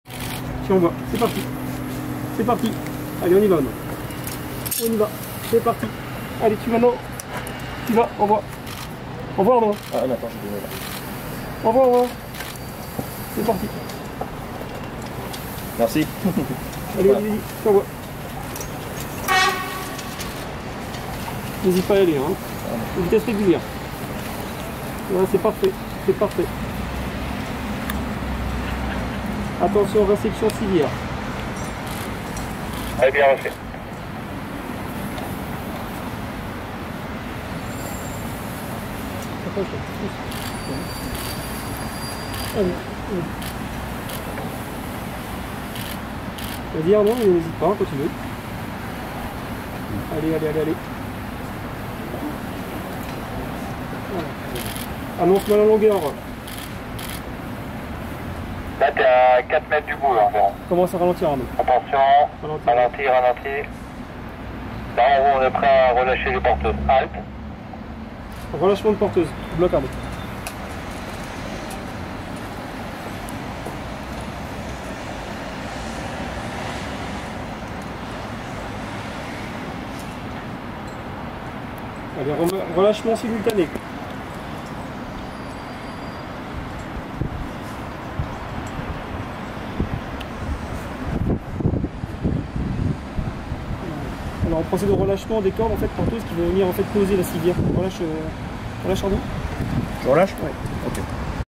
Opération de sauvetage au sommet de la cathédrale de Quimper
Les sapeurs-pompiers ont effectué une évacuation du haut d'une des tours de la cathédrale de Quimper. Un exercice qui consiste à faire descendre la victime dans une tyrolienne à plus de 50 mètres au dessus du sol.